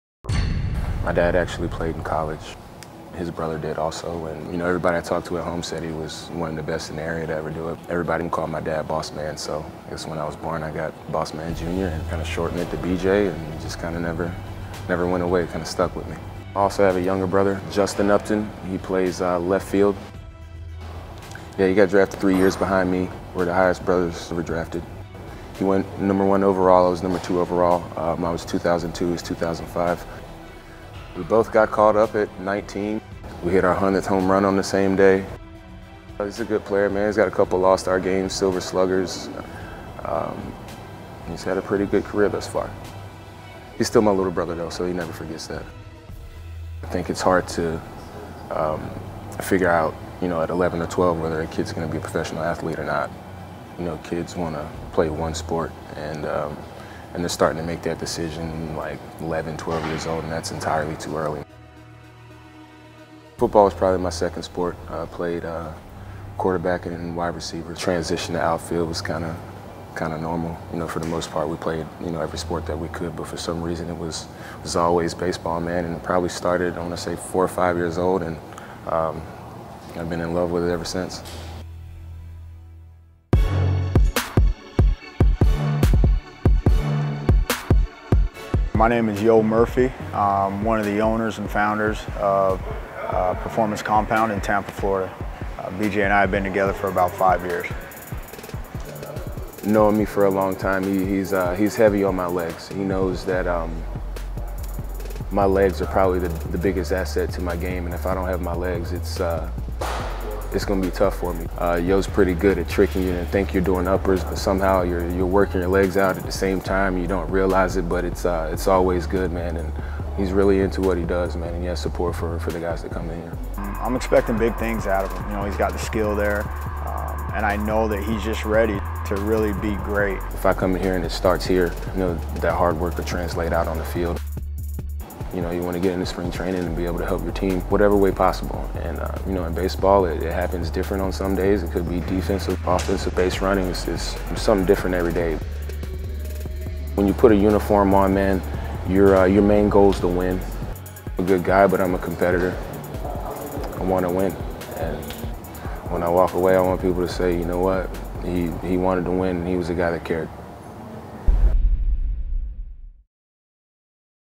Melvin Upton Jr 's Competitive Mindset Has Him Poised for Greatness This MLB Season
Melvin Upton Jr. (formerly known as B.J. Upton) talks about following in his father's footsteps, playing in the big leagues with his younger brother and what it takes to train for a 162-game season.